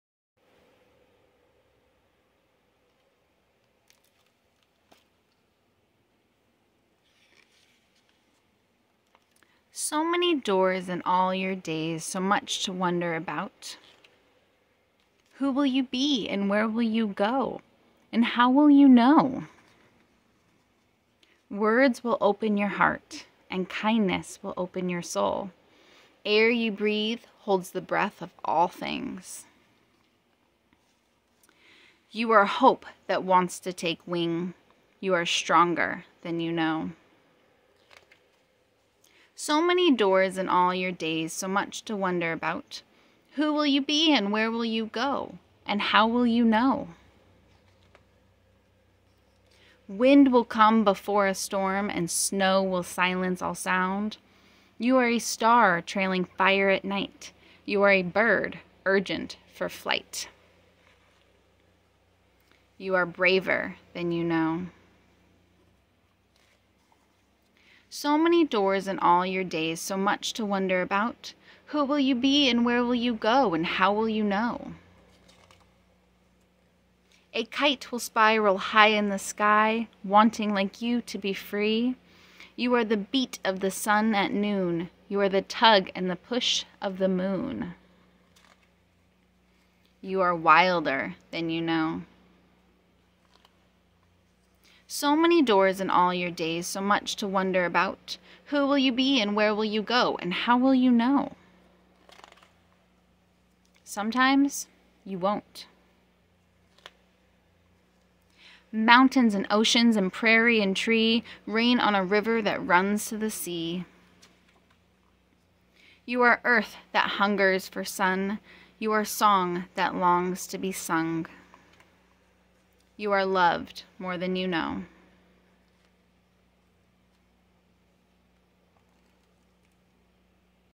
So Many Days by Alison McGhee
Read aloud the children's book, "So Many Days" by Alison McGhee